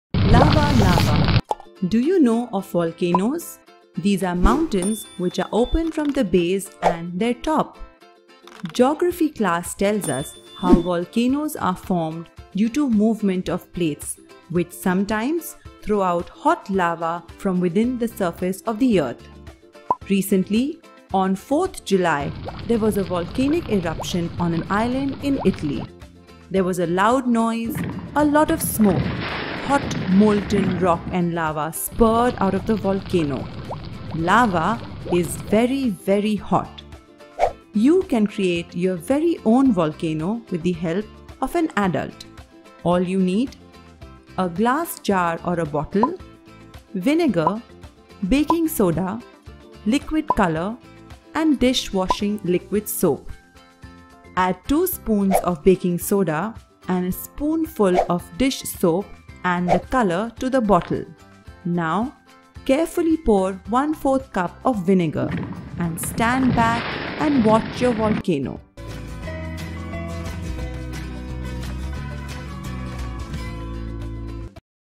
Volcanic Eruption | Learning Video For Kids | Foonaapp
Watch and Learn about VOLCANOES, how does a volcano ERUPTS, what is LAVA, how HOT can a volcano gets, where was the recent volcano eruption, Italy, how are volcanoes formed, different shapes of volcanoes.

Volcanic Eruption | Learning Video For Kids

Many more videos like this are available on our app 'Foona'